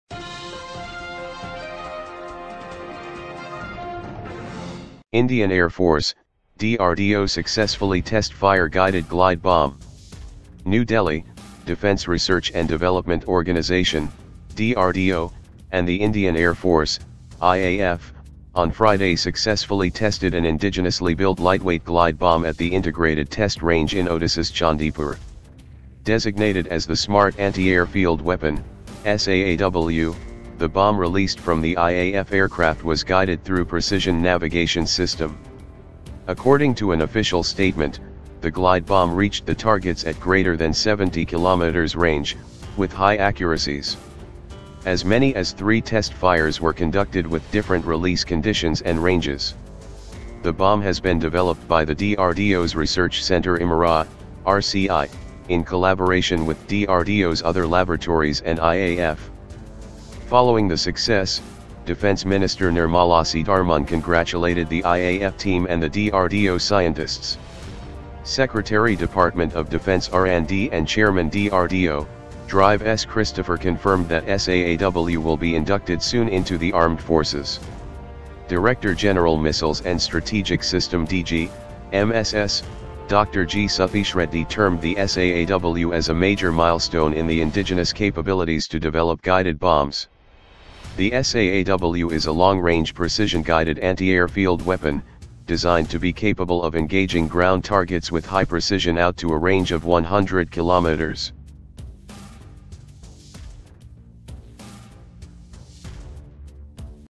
Indian Air Force, DRDO successfully test fire guided 'Glide Bomb'
NEW DELHI: Defence Research and Development Organisation (DRDO) and the Indian Air Force (IAF) on Friday successfully tested an indigenously-built lightweight 'glide bomb' at the Integrated Test Range in Odisha's Chandipur.

Designated as the Smart Anti-Airfield Weapon (SAAW), the bomb released from the IAF aircraft was guided through precision navigation system.